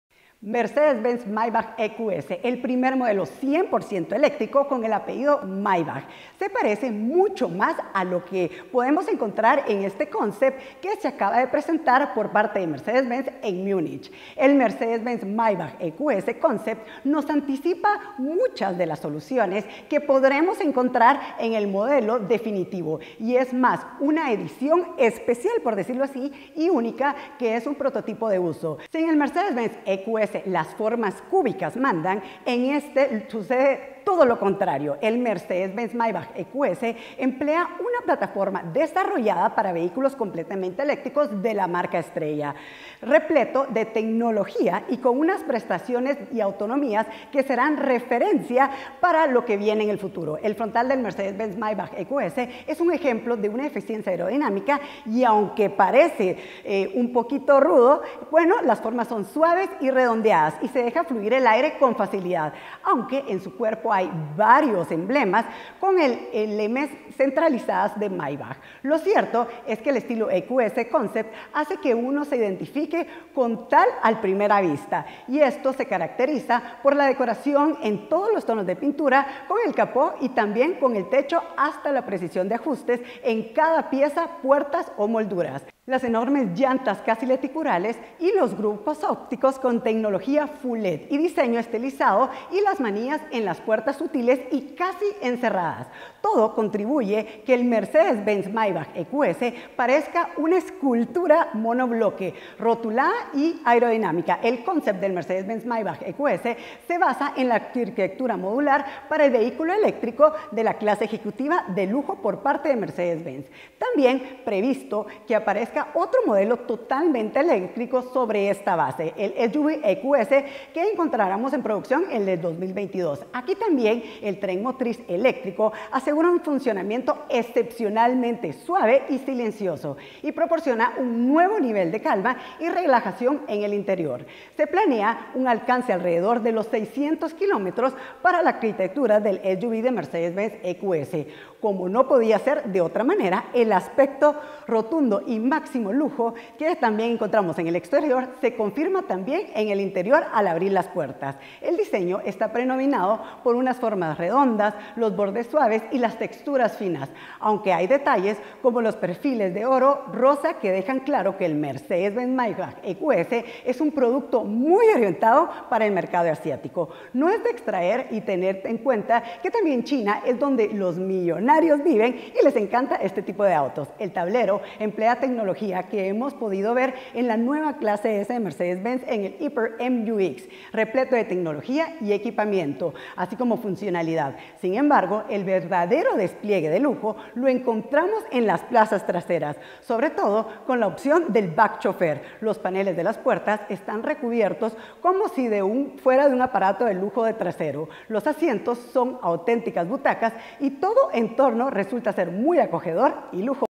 CONCEPT MERCEDES MAYBACH EQS
1: CONCEPT MERCEDES MAYBACH EQS
El primer modelo 100% eléctrico con el apellido Maybach se parecerá mucho más de lo que creemos a este concept car que se acaba de presentar Mercedes-Benz. El Mercedes-Maybach EQS Concept nos anticipa muchas de las soluciones que podremos encontrar en el modelo definitivo y es más una edición especial única que un prototipo al uso. Si en el Mercedes EQG las formas cúbicas mandan, en éste sucede todo lo contrario.
El Mercedes-Maybach EQS emplea la plataforma desarrollada para vehículos eléctricos de la marca de la estrella, repleto de tecnología y con unas prestaciones y autonomía que serán una referencia en la categoría.
El frontal del Mercedes Maybach EQS es un ejemplo de eficiencia aerodinámica y, aunque parece mastodóntico y rotundo, sus formas suaves y redondeadas dejan fluir el aire con facilidad. Aunque en su cuerpo hay varios emblemas con las emes entrelazadas de Maybach, lo cierto es que el estilo del EQS Concept hace que uno lo identifique como tal al primer golpe de vista, desde su característica decoración en dos tonos de pintura con el capó a juego con el techo hasta el preciso ajuste de cada pieza, puerta o moldura.